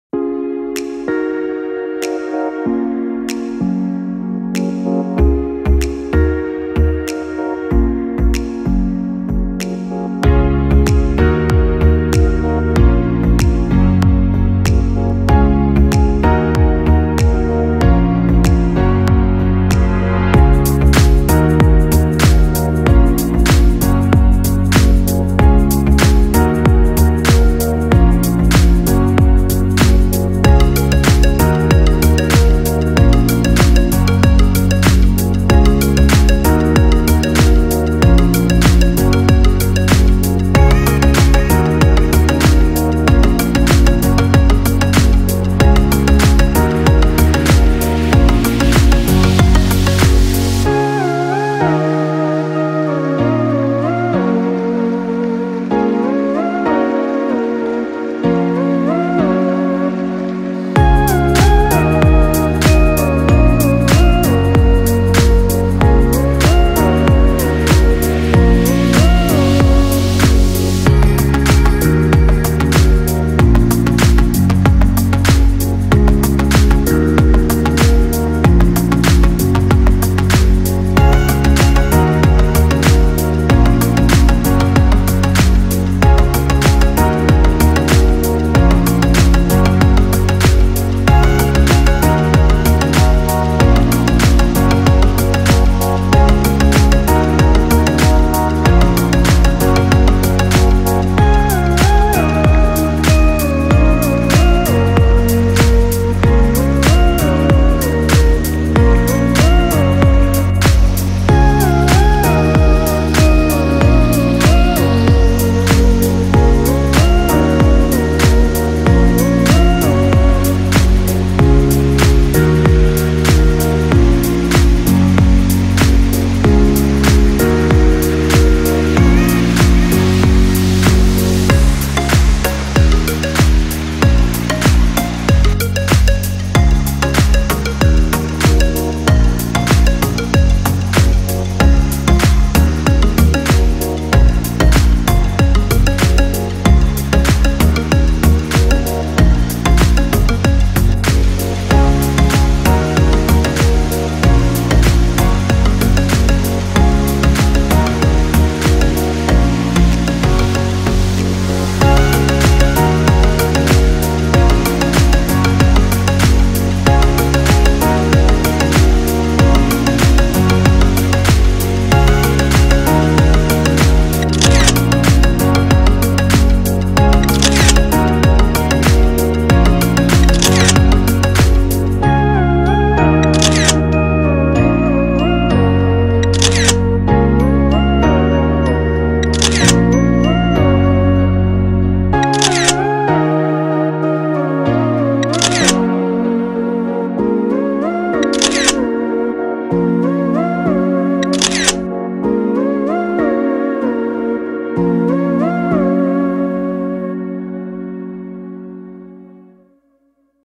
Des de la platja d'Espolla al salt de Martís
Un vol des de la Platja d'Espolla al salt de Martís, ple d'aigua de "gom a gom"